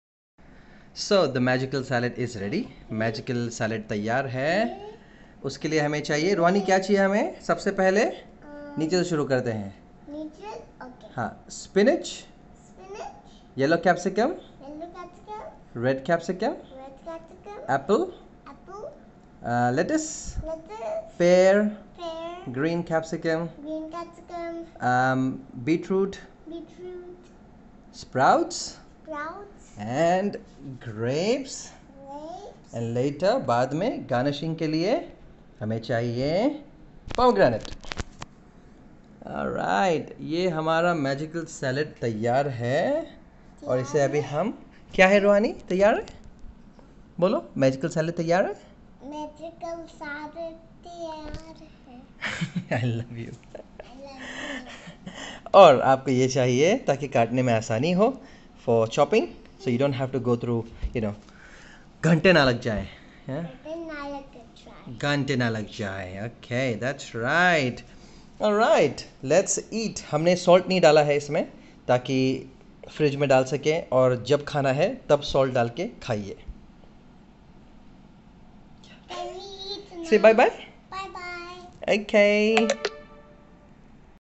Roohani prepares Magical salad with daddy for weight loss
Magical salad for weight loss.
salad for weight loss.
salads for everyone
yummiest salads
quick salads
super salads
super food